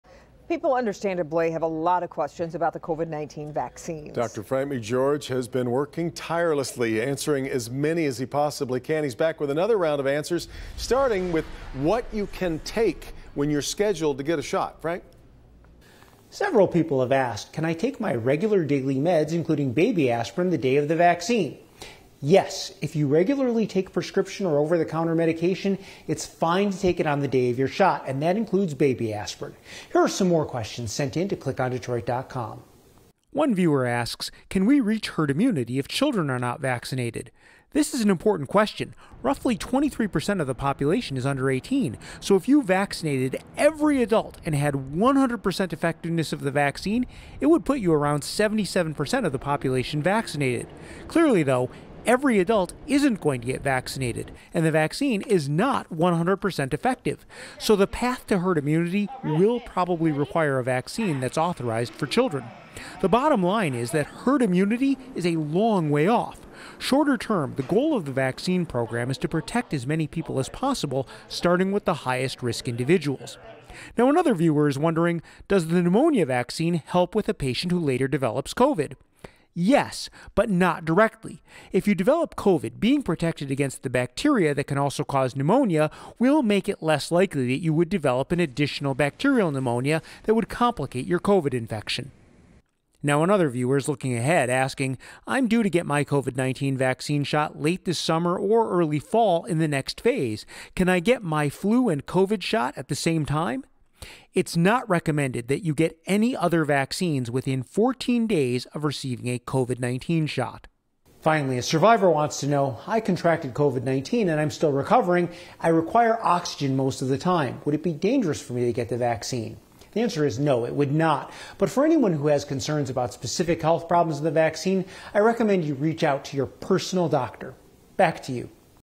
Answering COVID vaccine questions: Can we reach herd immunity if kids don’t get the vaccine? Can...
Many people have questions about the COVID-19 vaccine as it’s being rolled out to more people in Michigan.